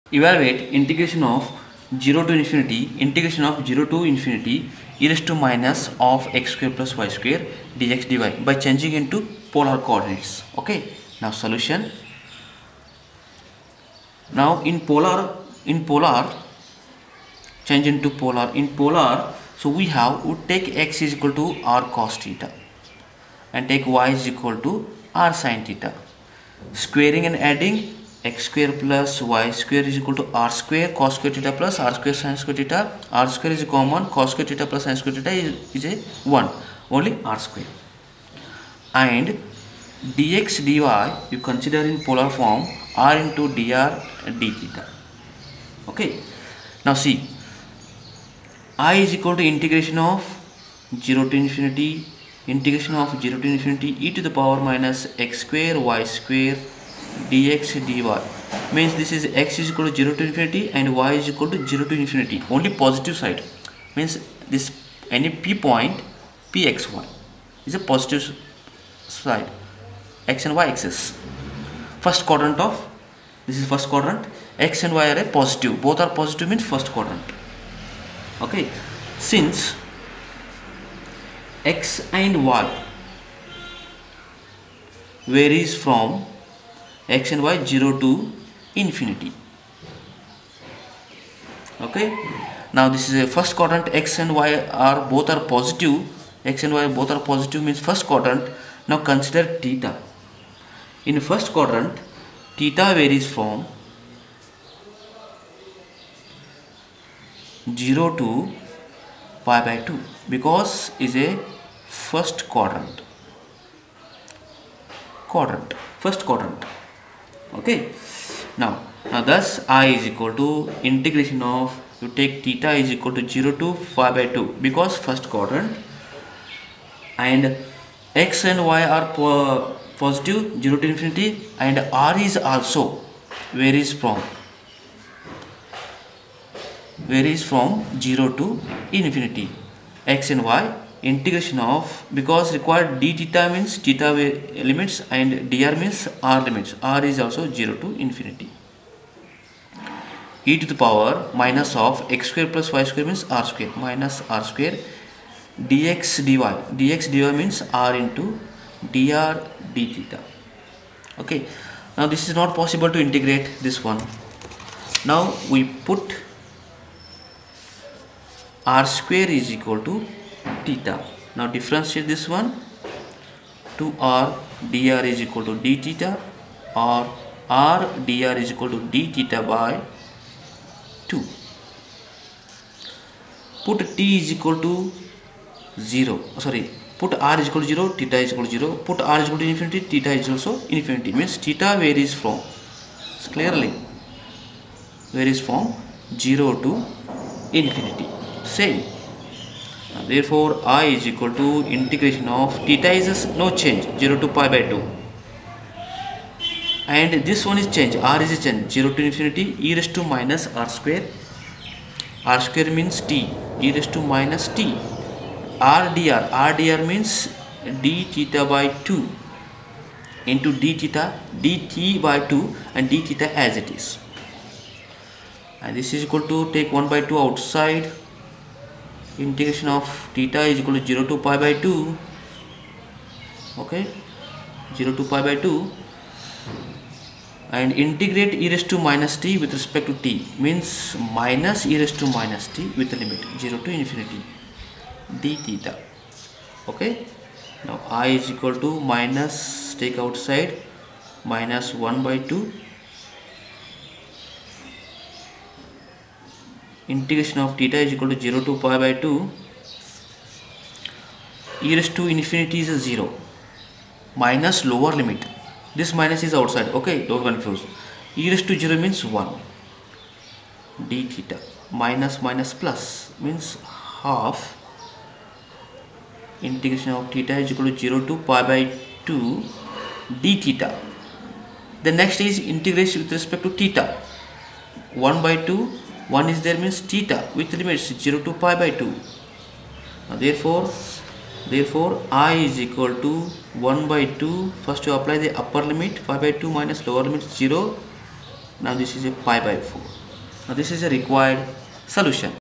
VTU Engineering Maths 1 Change into Polar coordinates good example(PART-9)
In this video explaining double integration change into polar coordinate. Changing into polar coordinates is a technique used to evaluate integrals in two dimensions by using polar coordinates instead of Cartesian coordinates.